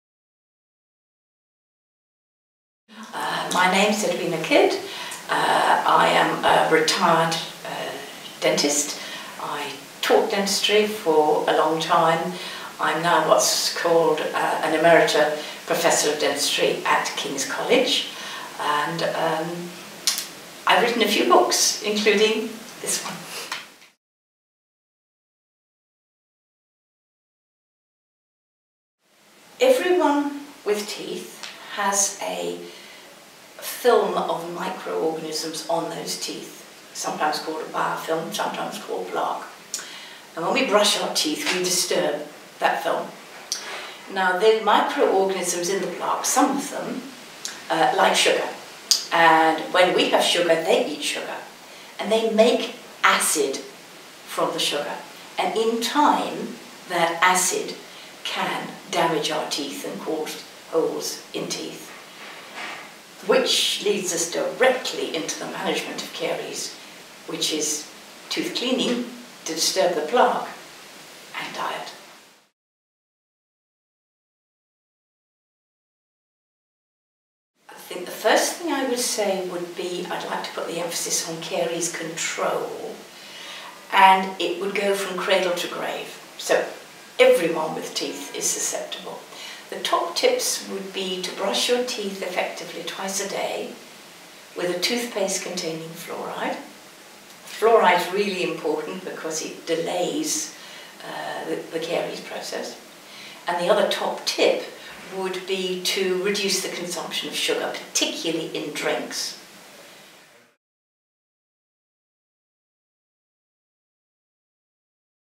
What is Dental Caries?
Professor Edwina Kidd talks about what dental caries is, and the top tips for preventing caries.

Professor Kidd is co-author of the fourth edition of Essentials of Dental Caries, which provides readers with an up-to-date, clinically relevant guide to dental caries. Written in an accessible style, the authors explain the biological and socioeconomic background of lesion development and progress.